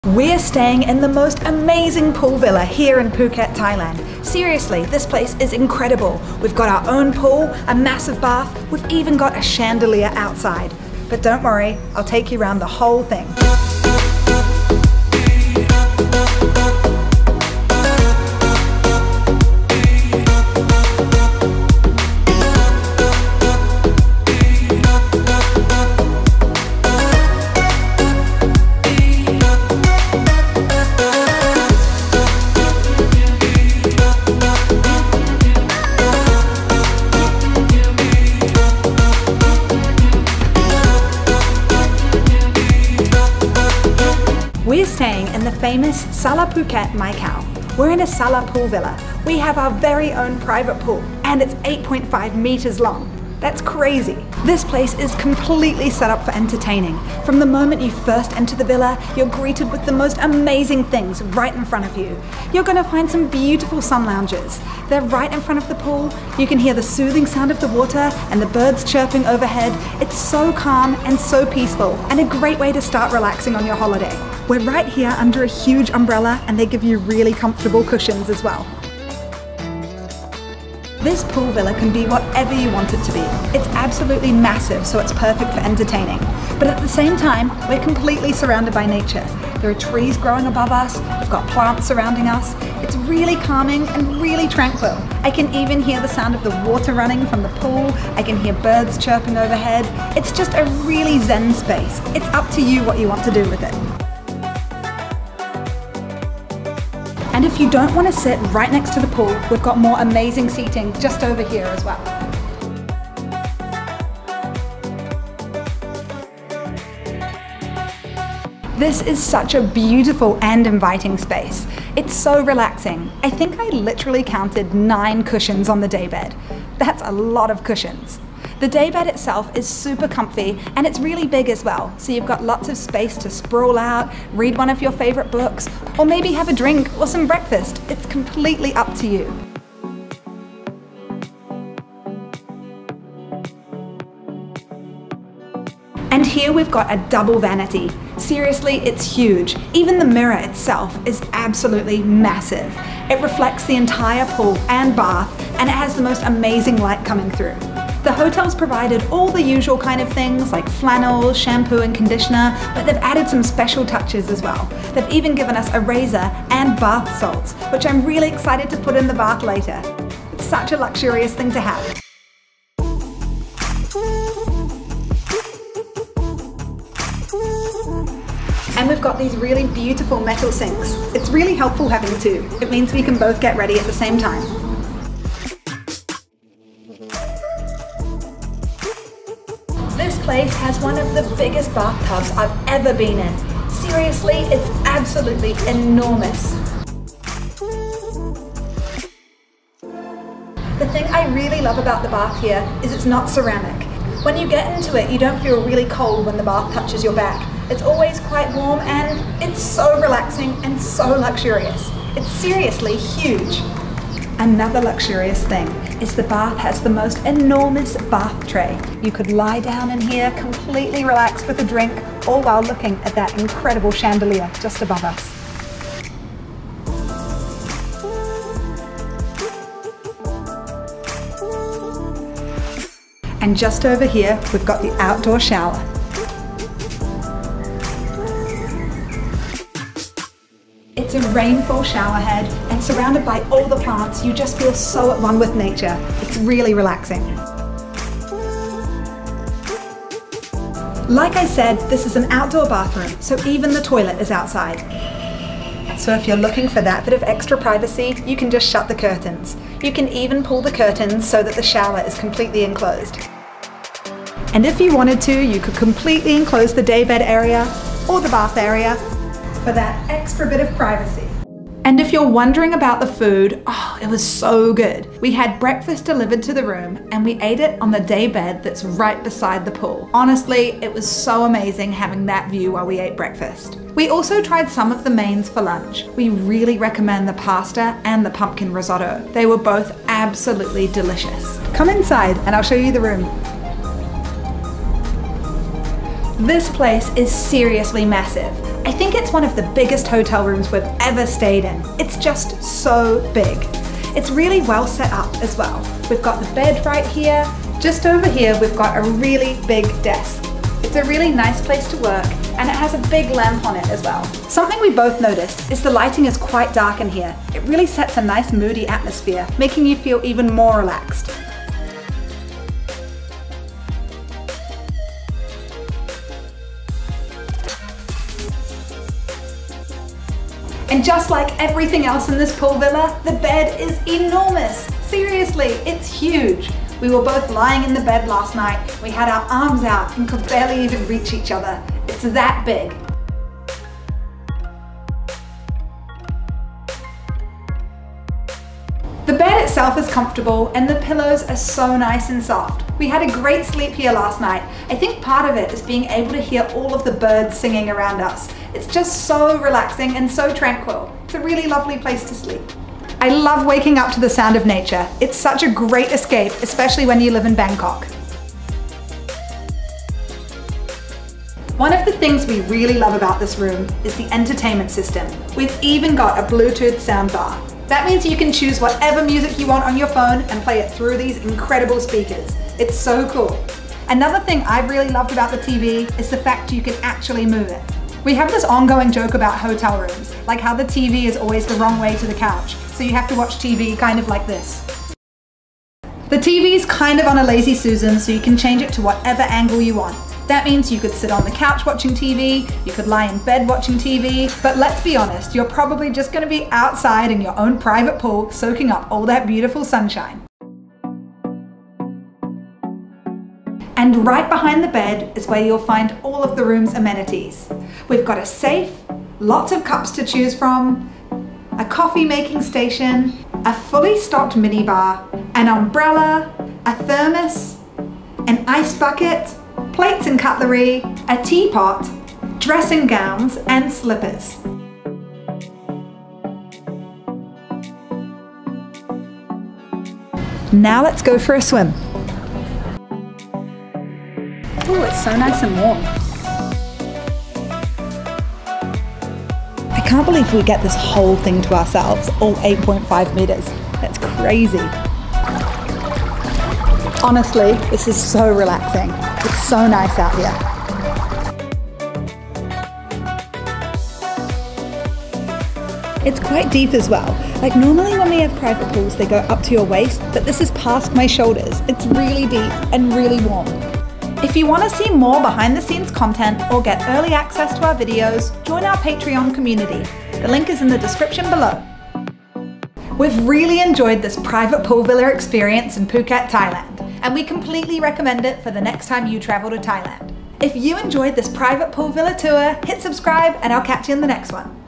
Sala Phuket - Luxury Private Pool Villa Tour in Thailand
Thailand is full of Private Pool Villas and we wanted to share our experience in this luxury hotel review. We stayed in a Sala Villa at The Sala Phuket Mai Khao Beach Resort in Phuket Thailand. If you are looking at where to go in Thailand Honeymoon the Phuket Private Pool Villa is an option you must consider. You don't need to be a Luxury Travel Expert to see why this Private Pool Villa is such a great choice when you next Travel Thailand.

Where do we get our music? All of the music in this video came from HERE (Try it for free)